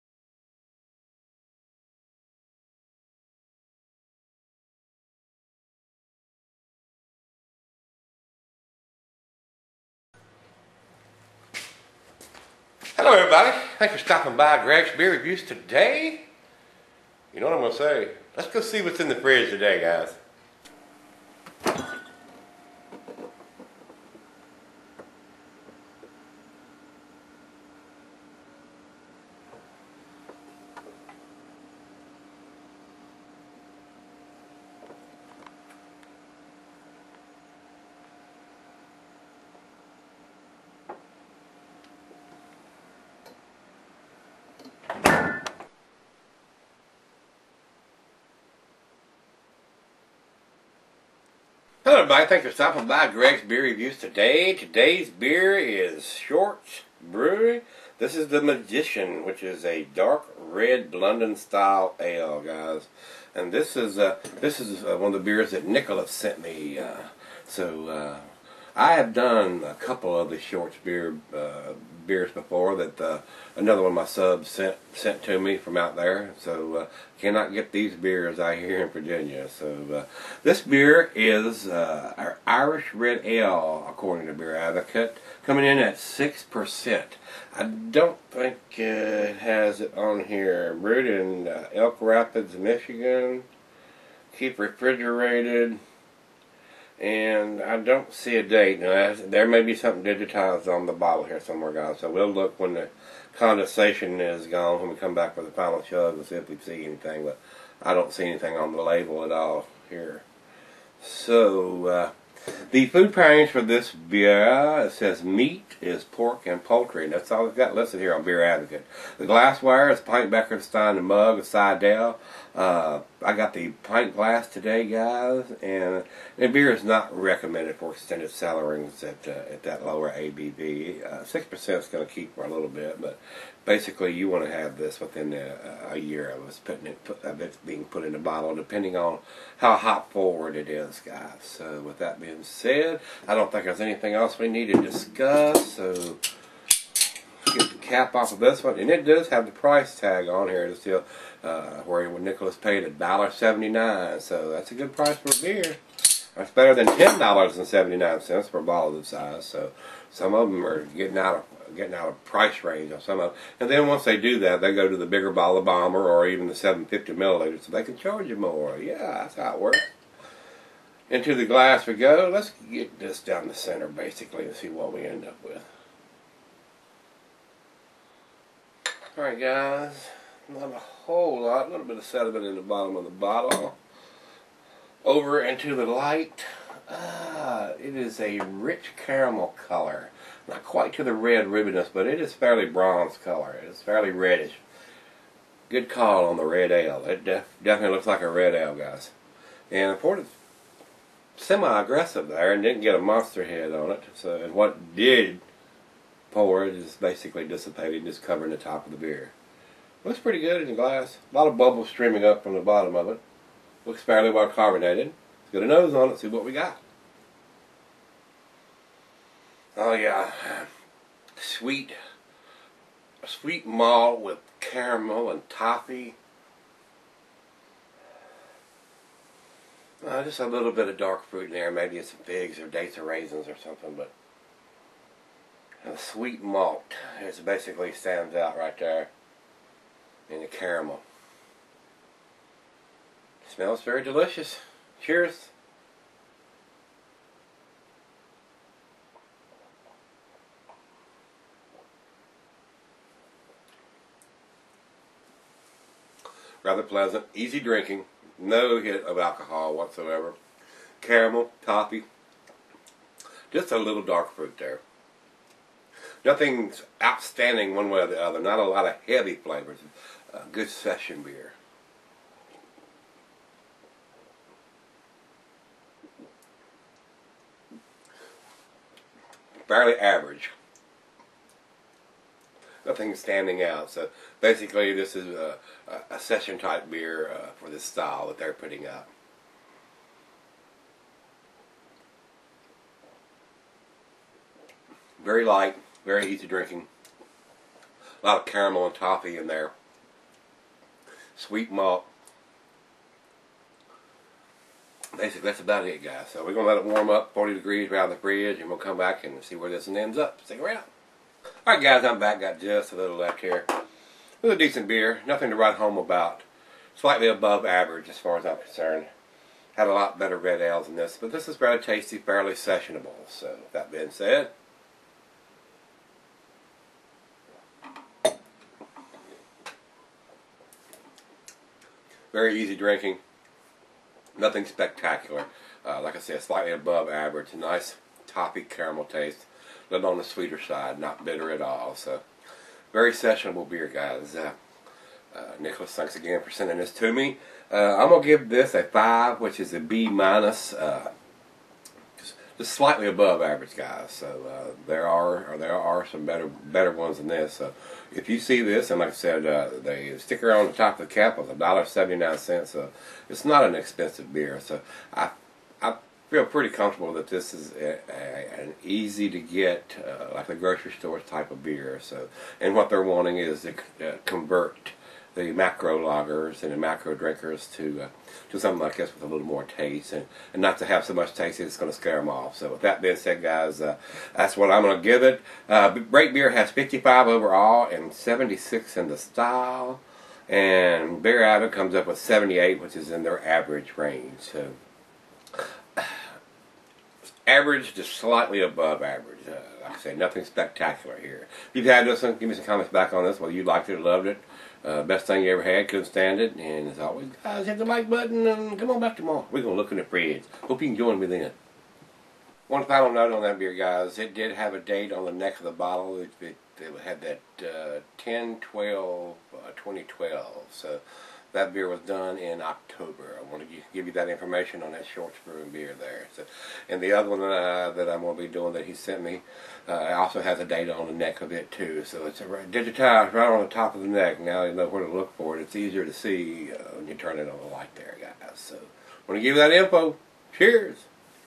Beer Review # 918 Shorts Brewery The Magician
Shorts Brewery The Magician     is the beer reviewed -
BEER SCALE
1- UNDRINKABLE
2- F
3- D
4- C
5- B-
6- B
7- B+
8- A-
9- A
10- A+
CHEERS!